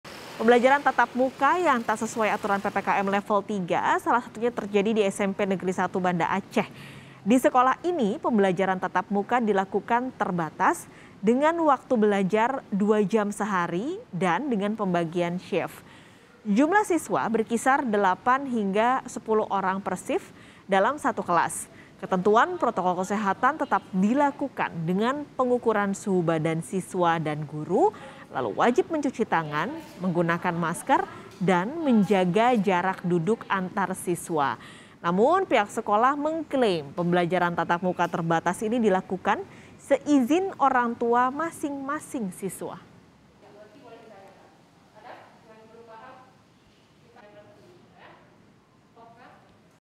Sejumlah Sekolah Nekat Belajar Tatap Muka Saat PPKM Level 3
KOMPAS.TV - Sejak 3 Agustus 2021, pemerintah menetapkan Aceh sebagai wilayah yang harus menerapkan PPKM Level 3. Namun, sejumlah sekolah di Banda Aceh menggelar belajar tatap muka di sekolah.

Salah satunya terjadi di SMP Negeri 1 Banda Aceh, di mana belajar tatap muka dilakukan terbatas dengan waktu belajar 2 jam sehari serta adanya pembagian shift. Jumlah siswa berkisar 8 hingga 10 orang per shift dalam satu kelas.

Ketentuan protokol kesehatan tetap dilakukan dengan pengukuran suhu badan siswa dan guru, wajib mencuci tangan, pakai masker, dan menjaga jarak duduk antar siswa.

Pihak sekolah mengklaim bahwa pembelajaran tatap muka terbatas ini dilakukan seizin orangtua masing-masing siswa.

Padahal dalam aturan, wilayah PPKM Level 3 dan 4 tidak diizinkan melakukannya dan semua pembelajaran wajib dilakukan secara online.